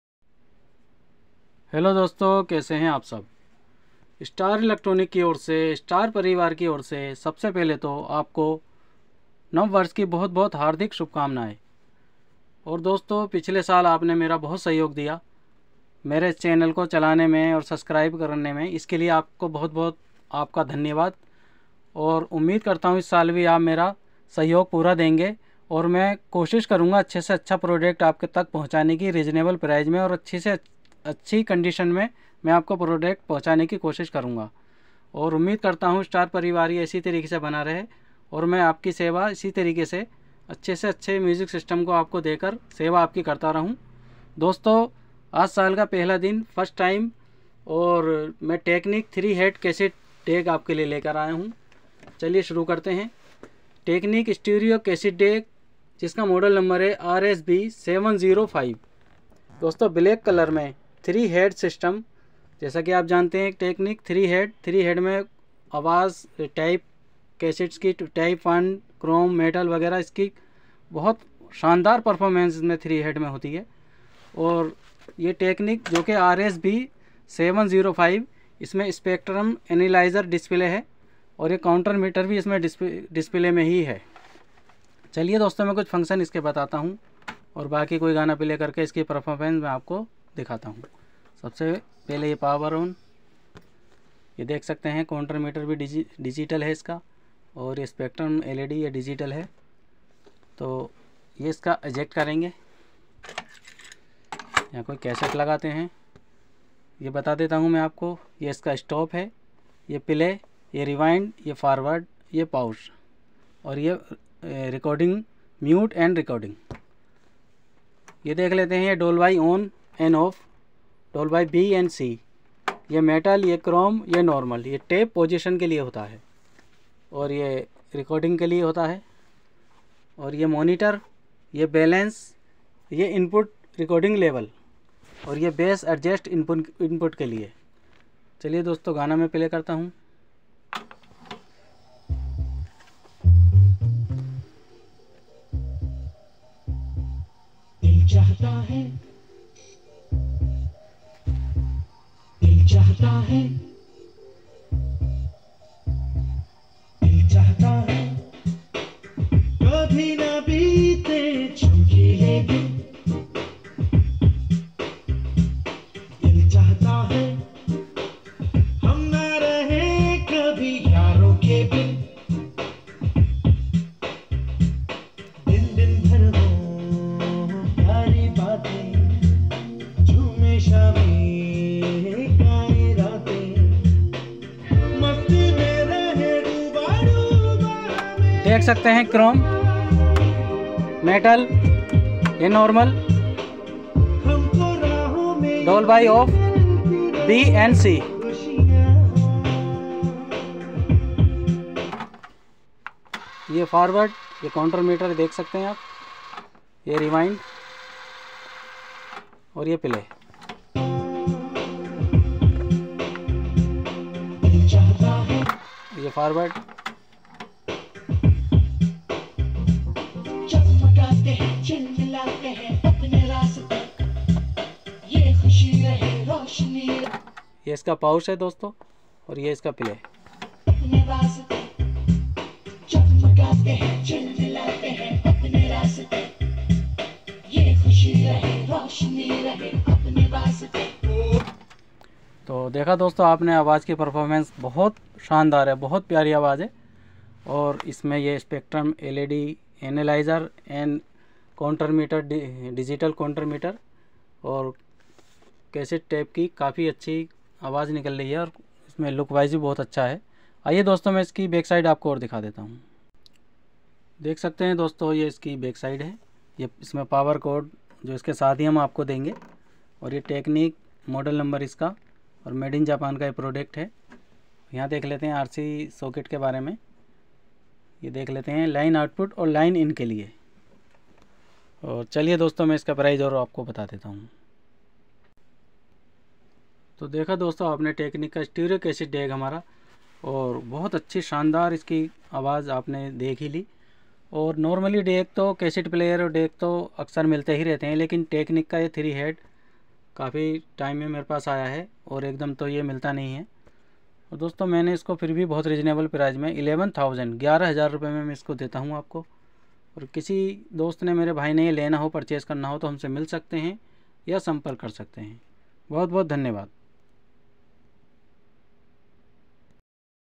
Technics 3 HEAD CASSETTE DECK Very Nice Sound Quality 👍 Price - 11000/- Only
FOR SALE

Technics 3 HEAD STEREO CASSETTE DECK RS-B705

Very Nice Sound performance 👍